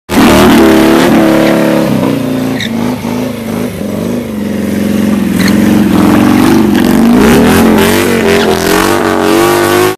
UNKNOWN INDUSTRIES mowing the lawn
justin mowing the lawn.

Watch the Harley Wheelies x UNKNOWN Industries - 13th Level - DVD Trailer featuring Nick Leonetti and Buddy Suttle

youtube harley wheelies!!!